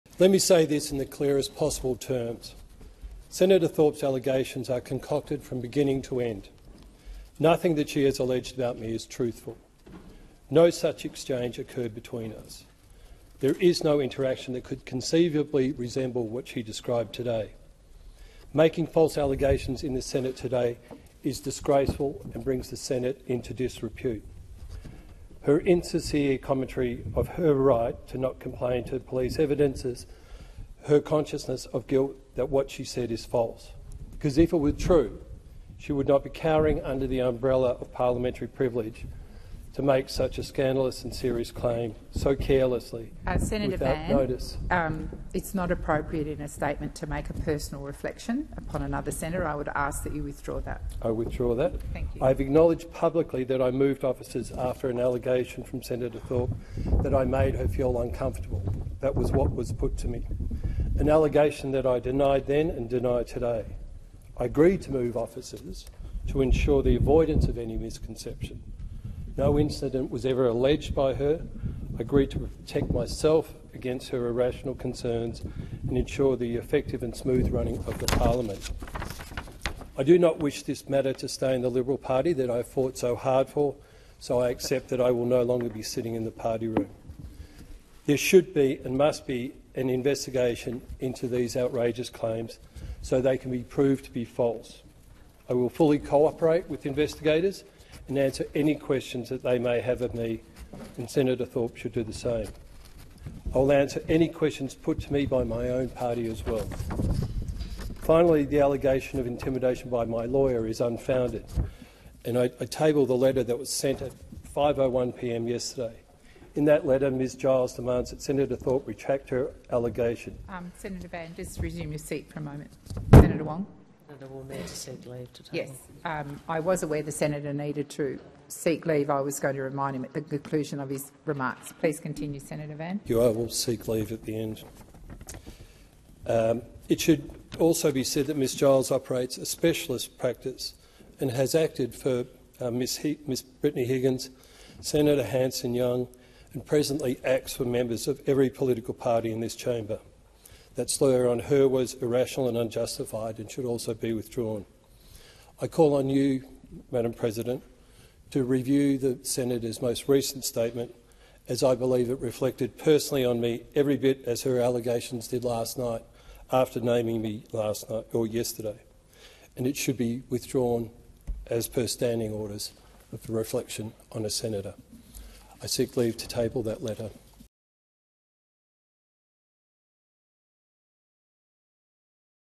'Concocted from the beginning': Senator David Van denies Lidia Thorpe's allegations
Liberal Senator David Van continues to strenuously deny Senator Lidia Thorpe's allegations against him.